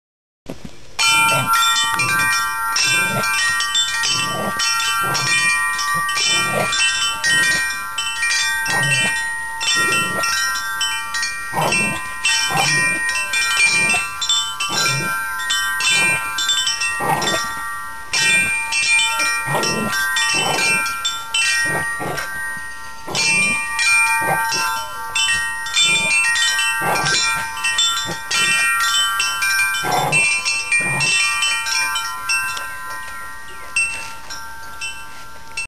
Border Collie Tootsie Rings Her Wind Chimes
Tootsie likes to ring her chimes. No one taught her how to do it, she just started ringing them one day to alert us that people were coming up the driveway. Now she rings them whenever she goes outside to let the valley know that she is on watchdog duty. She rings them at other dogs and horses that go by the property. She also uses her chimes to let everyone know when it's dinner time!  In other words, she rings her chimes, on and off, all day long.